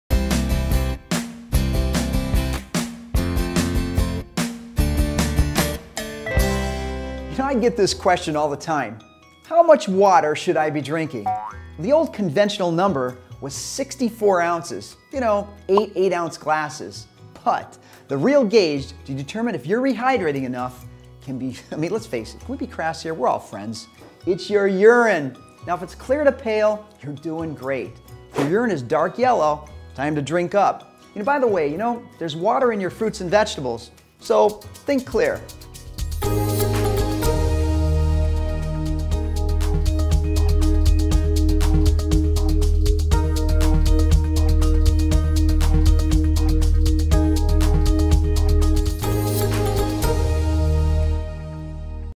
Jaime Brenku Lite Bites: How Much Water Should You Be Drinking?
In this video, Evergreen Wellness® Fitness Coach Jaime Brenkus discusses how much water to drink daily to stay hydrated.

This video is just one of many that offers health and wellness tips

People are always asking Jaime how much water they should drink daily. Conventional wisdom says 64 ounces or, eight 8-ounce glasses. But the real gauge to determine whether you’re hydrating enough is your urine. If it’s clear to pale, you’re okay. If it’s dark yellow, it’s time to drink more water. Also, remember that there’s water in fruits and vegetables so, think clear.

Socialize with us. It’s good for you!